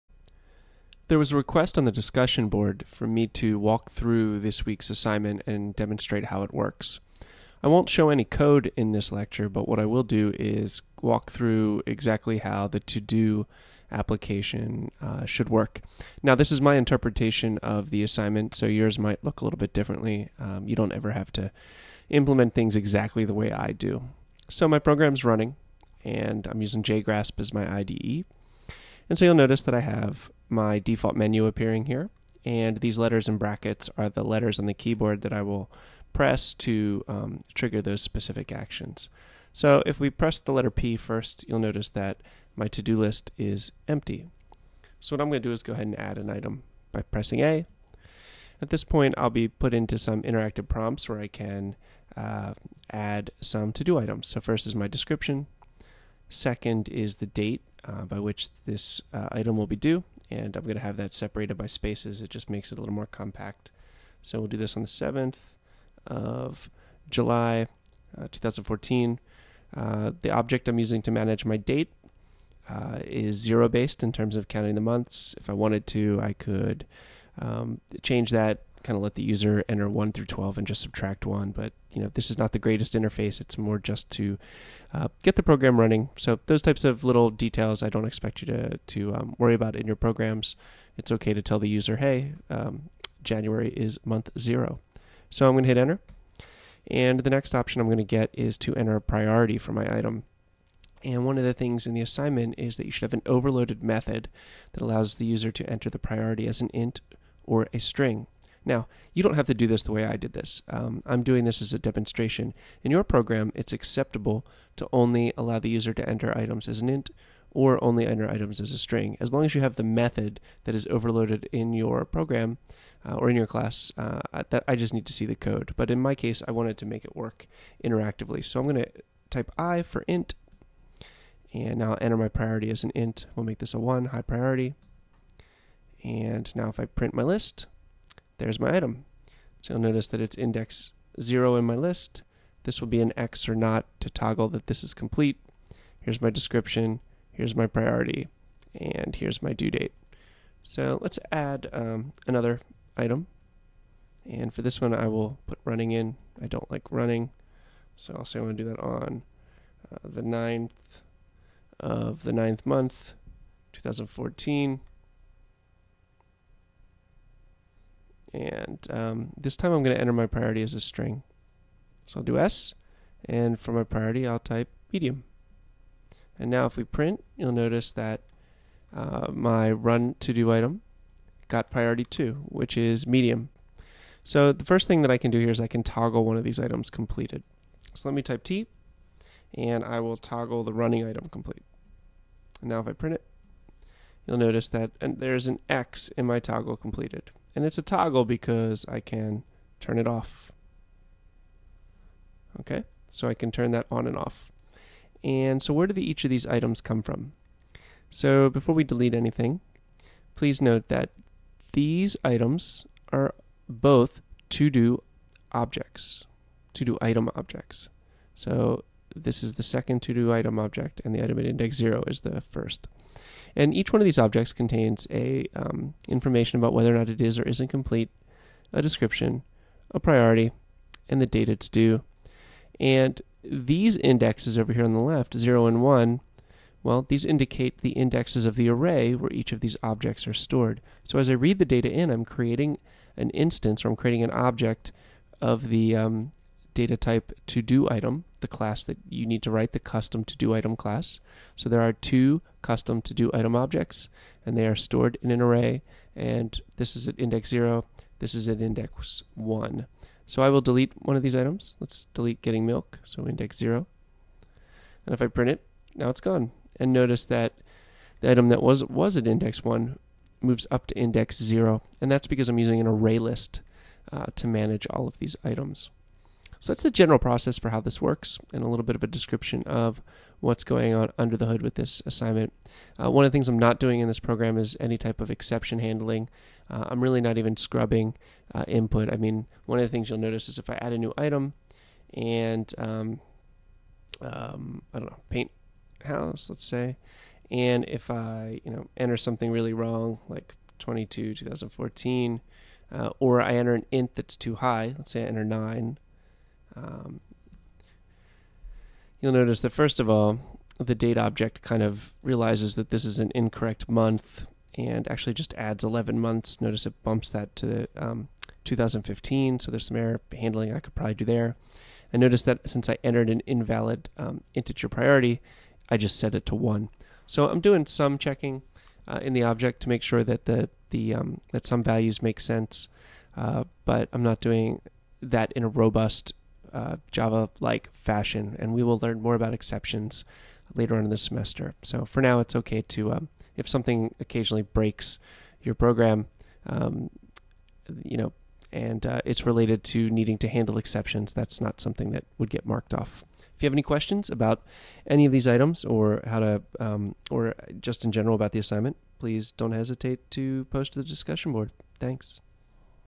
ToDo Assignment Demo
Walkthrough of ToDo Assignment for my online OO course.